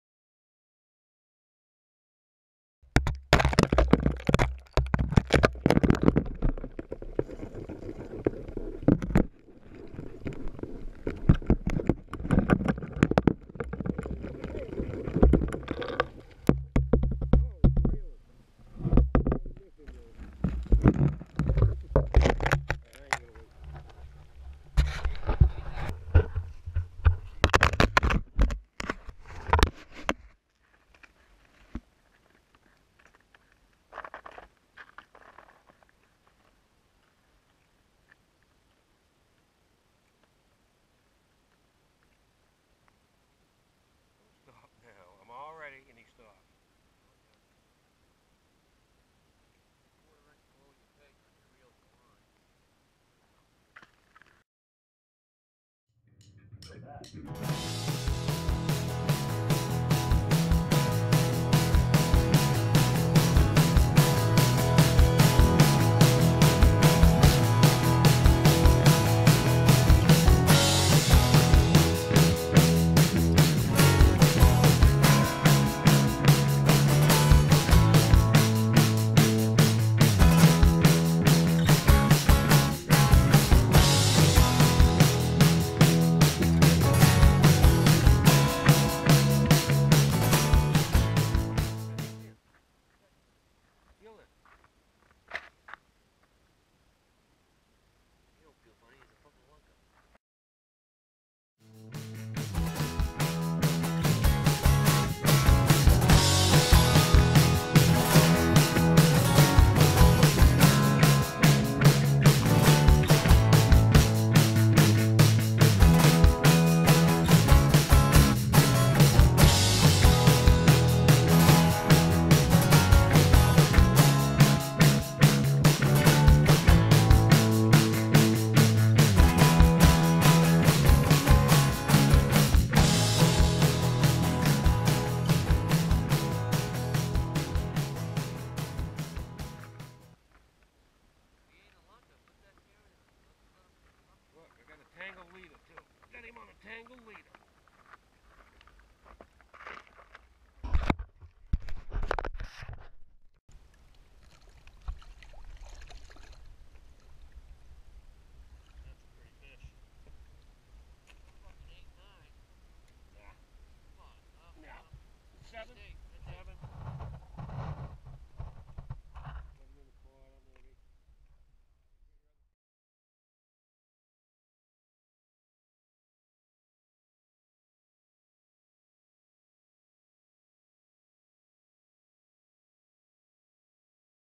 Jack Traps East Grand 2015 - Just a Taste
Teaser video of Jack Traps Ice Fishing Outfitters 2015 East Grand Trip catching big lake trout! Here is a Tim Jack catching a nice one!